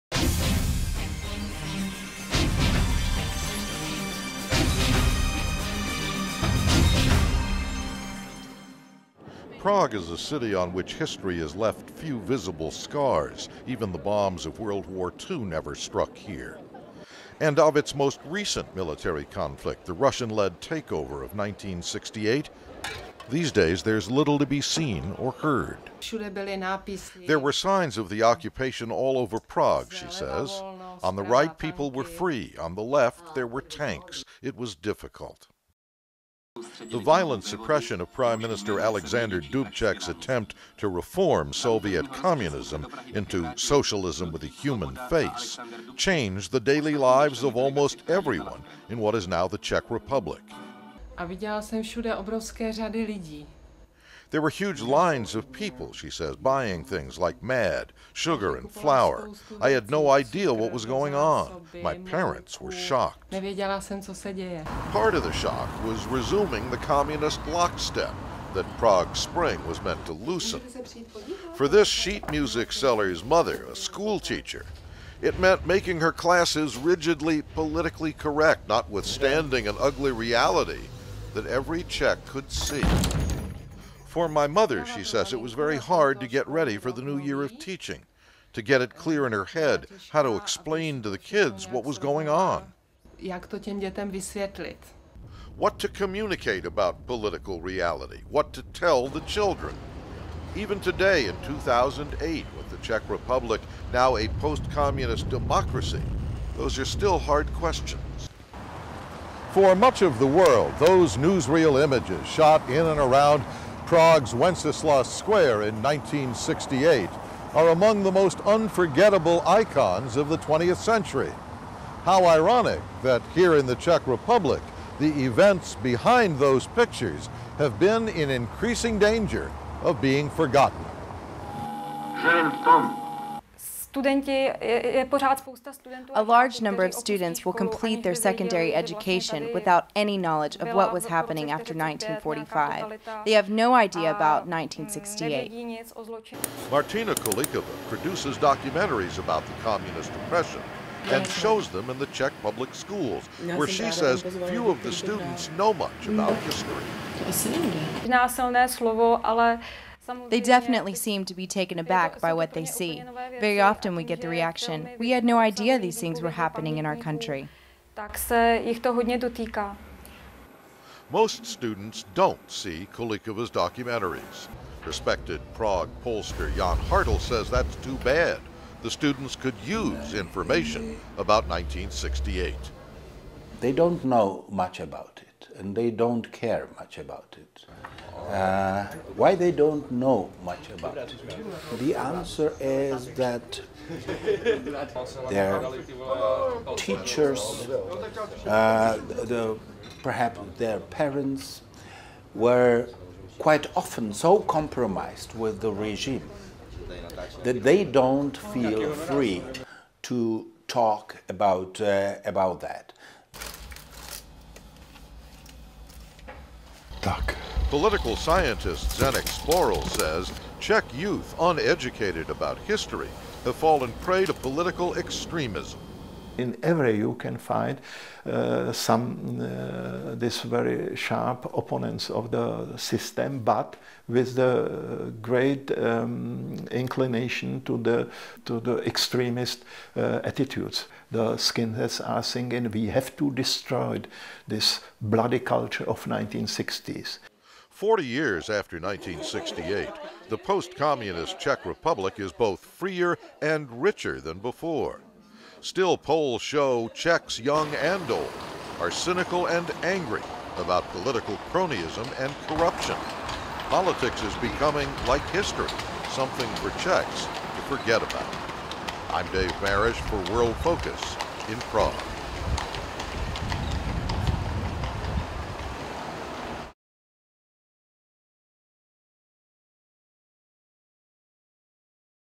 Czech youth unware of Soviet past
Forty years ago, Russia invaded the former Czechoslovakia to resume communist rule. The invasion launched demonstrations in the streets of Prague to challenge Soviet oppression.

Today, the younger generation of Czech citizens know little about the invasion and subsequent demonstrations. It poses a problem for schools and families to talk about their countrys political past.

Worldfocus correspondent Dave Marash reports on the forgotten history of the 1960s in present-day Czech Republic.